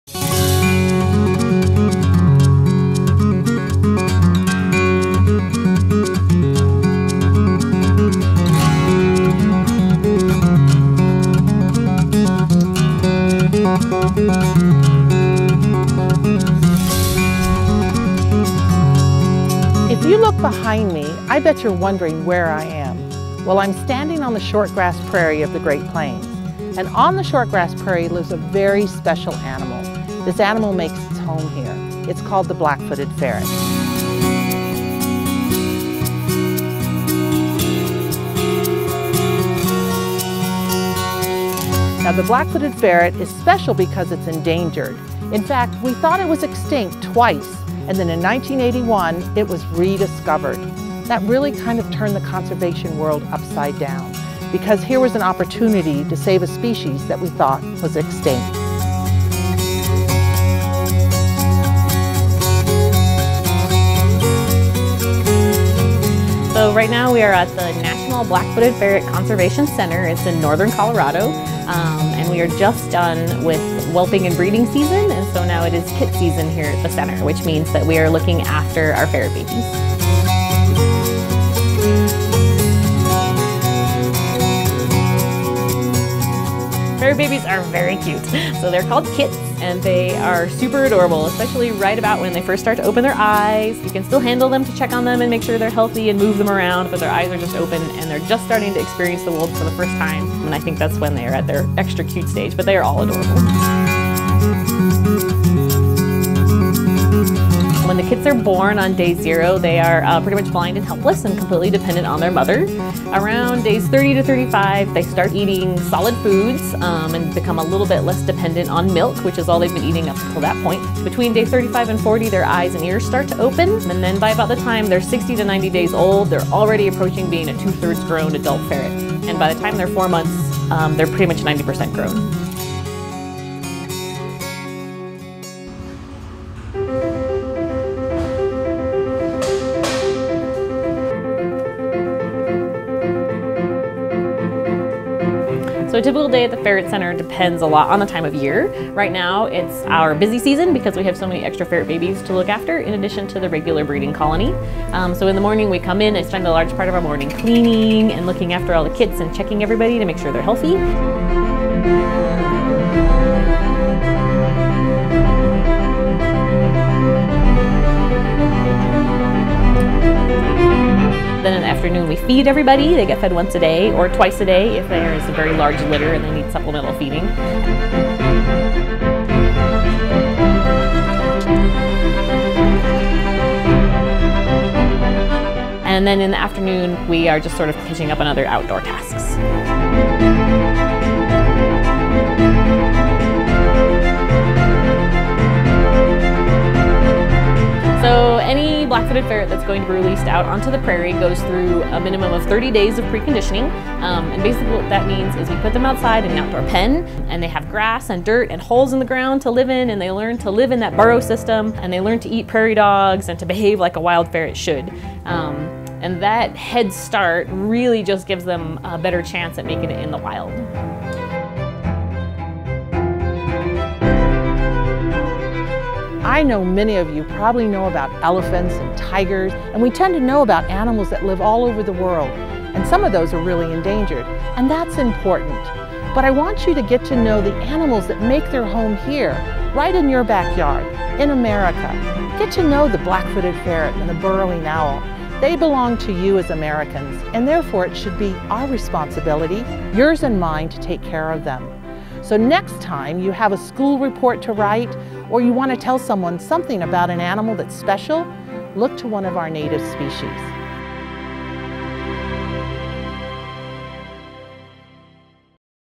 Black Footed Ferrets
The black-footed ferret was thought to be extinct--twice--but was rediscovered in 1981. See what's being done to ensure its survival.